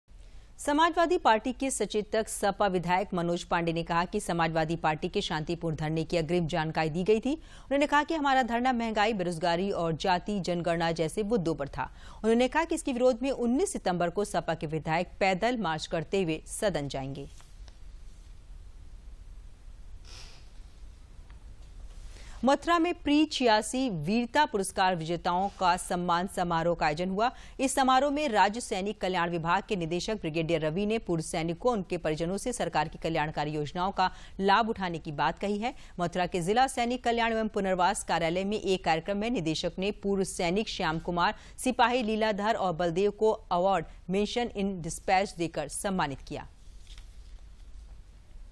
सपा के विधायक Manoj Pandey ने बैठक की
समाजवादी पार्टी के सचेतक सपा विधायक मनोज पांडेय ने कहा कि समाजवादी पार्टी के शांतिपूर्वक धरने की अग्रिम जानकारी दी गयी थी। उन्होंने कहा कि हमारा धरना मंहगाई बेरोजगारी और जातीय जनगणना जैसे मुद्दों पर था। उन्होंने कहा कि इसके विरोध में 19 सितंबर को सपा के विधायक पैदल मार्च करते हुए सदन जायेंगे।